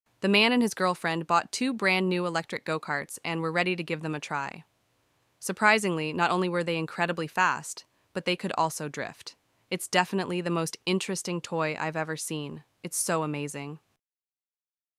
Go cart for adults is fun af
The man and his girlfriend bought two brand-new electric go-karts and were ready to give them a try. Surprisingly, not only were they incredibly fast, but they could also drift. It's definitely the most interesting toy I've ever seen. It's so amazing.